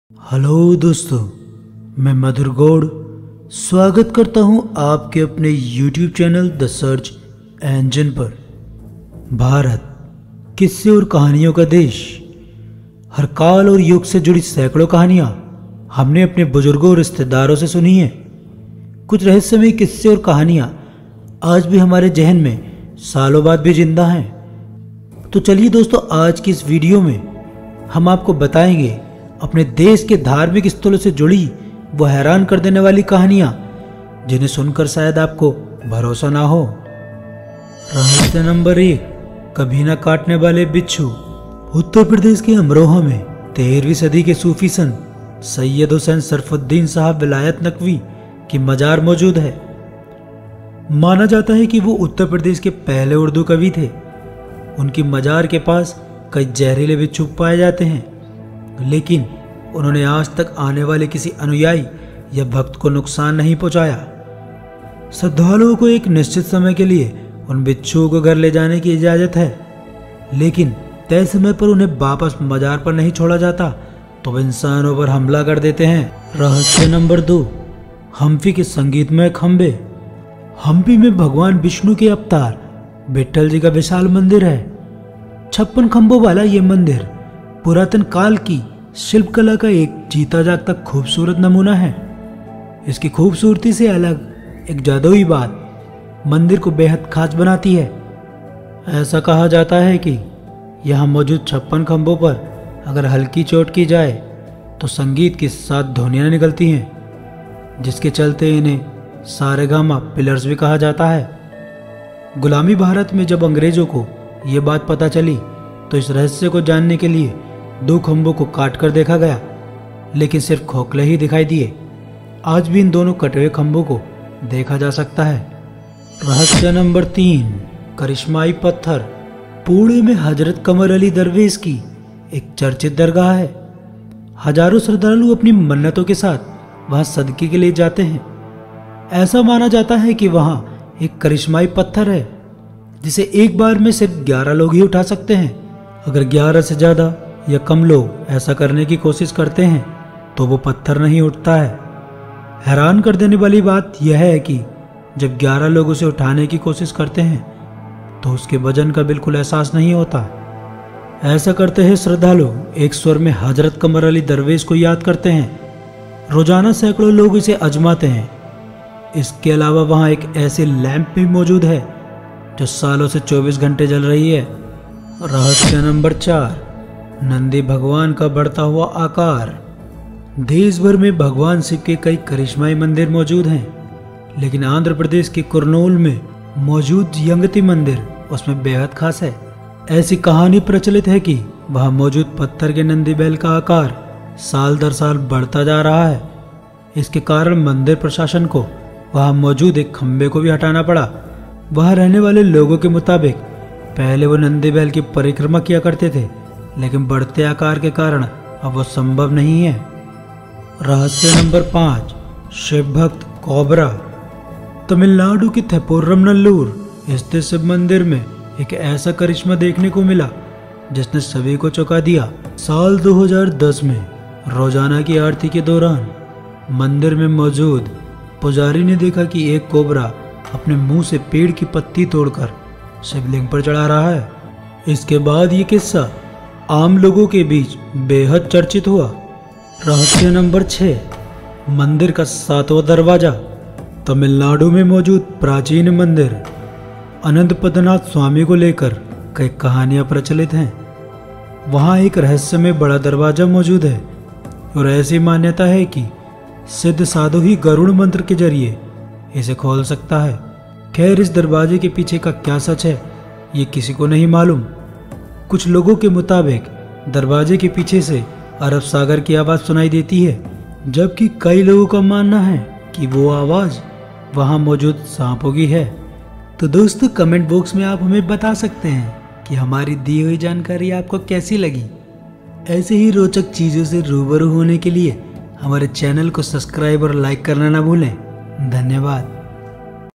अद्भुत और अनसुलझे रहस्य | THE SEARCH ENGINE | MADHUR GAUR | ADBHUT | UNSULJHE | RAHASAYA
अद्भुत और अनसुलझे रहस्य
यूपी के अमरोहा का हज़रत सैयद शाह साहब की दरगाह। यहां के बिच्छू लोगों को ना काटते हैं और ना परेशान करते हैं
U.P. Amroha hazarat Sayed shah sahab ki dargha .
हम्पी में संगीतमय स्तंभ
भगवान विष्णु जी के अवतार बिट्ठल जी का मंदिर ,यहाँ खम्बो से सा रे गए मा प् की आवाज आती है
मंदिर, जहां आज भी बढ़ रहा है मूर्ति का आकार
आंध्र प्रदेश के कुरनूल में श्री यंगती उमा महेश्‍वरा मंदिर नाम का एक मंदिर स्थित है। अपने आप में इस अनोखे मंदिर के बारे में कहा जाता है कि यहां नंदी की प्रतिमा के बढ़ते आकार की वजह से रास्‍ते में पड़ रहे कुछ खंबों को तक हटाना पड़ा और यह मूर्ति आज भी बढ़ रही है। ऐसे में एक-एक करके यहां नंदी के आस-पास स्थित कई खंबों को हटाना पड़ा गया है।
रहस्यमयी है सबसे अमीर पद्मनाभ मंदिर, चाबी नहीं मंत्रों से खुलता है दरवाजा
रहस्यमयी पत्थर
हजरत पीर कमर अली दुर्वेश की दरगहा